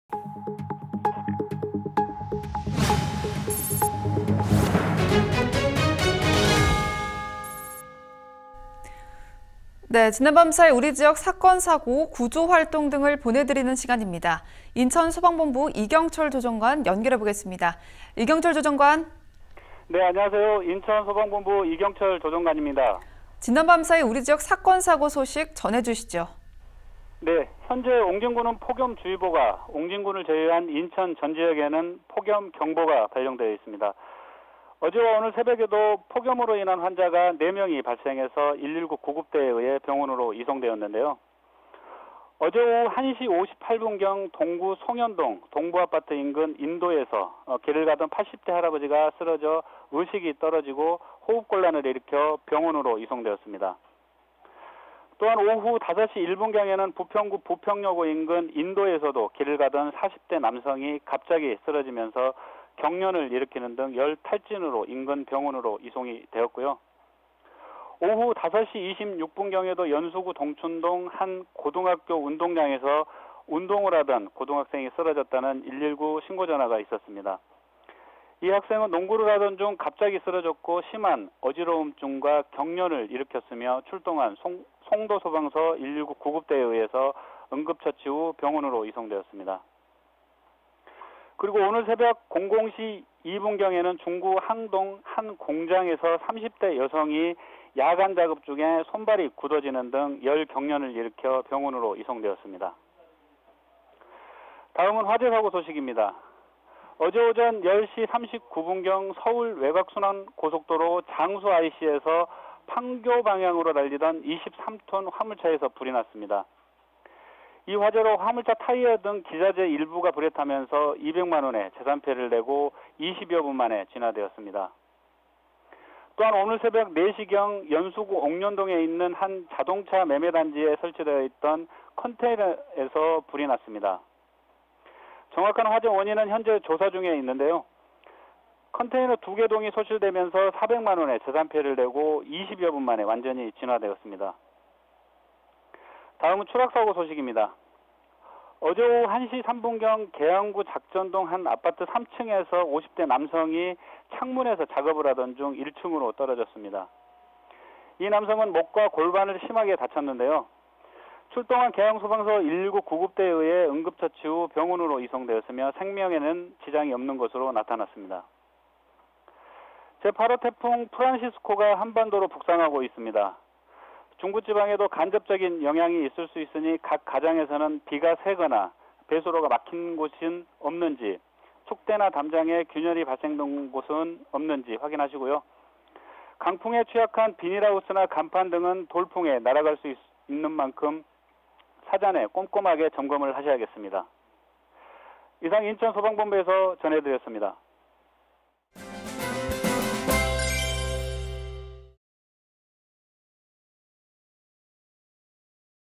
[인천소방브리핑] 8/06 폭염에 환자 4명 발생·달리던 화물차 화재
김다솜 아나운서 )
지난 밤 사이 우리지역 사건 사고 구조 활동 등을 보내드리
는 시간입니다.

인천소방본부 이경철 조정관 연결해보겠습니다. 이경철 조정관
나와주세요.

인천소방본부 이경철 조정관 )
네 안녕하세요. 인천소방본부 이경철 조정관입니다.

김다솜 아나운서 )
지난 밤 사이 우리지역 사건 사고 소식 전해주시죠.

현재 옹진군은 폭염주의보가 옹진군을 제외한 인천 전 지역에는 폭염 경보가 발령되어 있습니다.
어제와 오늘 새벽에도 폭염으로 인한 환자가 4명 발생해서 119구급대에 의해 병원으로 이송되었는데요.

어제 오후 1:58경 동구 송현동 동부아파트 인근 인도에서
길을 가던 80대 할아버지가 쓰러져 의식이
떨어지고 호흡곤란을 일으켜 병원으로 이송되었습니다.

또한 오후 5: 1분경에는 부평구 부평동 부평여고 인근 인
도에서도 길을 가던 40대 남성이 갑자기 쓰러지면서
경련을 일으키는 등 열탈진으로 인근병원으로 이송되었고요.

오후 5:26분경에도 연수구 동춘동 한 고등학교 운동장에서
운동을 하던 고등학생이 쓰러졌다는 119신고전화가 있었습니
다.
이 학생은 농구를 하던 중 갑자기 쓰러졌고 심한 어지러움과
경련을 일으켰으며 출동한 송도소방서 119구급대에 의해서
응급처치 후 병원으로 이송되었습니다.

그리고 오늘 새벽 00시: 2분경에는 중구 항동 한 공장에
서 30대 여성이 야간 작업 중에 손발이 굳어지고
열경련을 일으켜 병원으로 이송되었습니다.
다음은 화재 사고 소식입니다.
어제 오전 10:39분경 서울 외곽 순환고속도로 장수IC에
판교방향으로 달리던 23톤 화물차에서 불이 났습니다.
이 화재로 화물차 타이어 등 일부 기자재 불에 타면서
200만원에 재산피해를 내고 20여분만에 진화 되었습니다.

또한 오늘 새벽 4:00경 연수구 옥련동에 있는
한 자동차매매단지에 설치 되어있던 컨테이너에서 불이 났습니
다. 정확한 화재 원인은 현재 조사 중에 있는데요.
컨테이너 2개동이 소실되면서 400만원에 재산피해를 내고
20여분만에 완전히 진화되었습니다.

다음은 추락사고 소식입니다.
어제 오후 1시 3분경 계양구 작전동 한 아파트 3층에서
50대 남성이 창문에서 작업을 하던 중 1층으로 떨어졌습니
다.

이 남성은 목과 골반을 심하게 다쳤는데요.
출동한 계양소방서 119구급대에 의해 응급처치 후
병원으로 이송 되었으며 생명에는 지장이 없는 것으로 나타났
습니다.

제8호 태풍 프란시스코가 한반도로 북상하고 있습니다.
중부지방에도 간접적인 영향이 있을 수 있으니
각 가정에서는 비가 새거나 배수로가 막힌 곳은 없는지,
축대나 담장에 균열은 없는지 확인하시고요.
강풍에 취약한 비닐하우스나 간판 등은
돌풍에 날아갈 수 있는 만큼 사전에
꼼꼼하게 점검하셔야 겠습니다.
이상 인천소방본부에서 전해드렸습니다.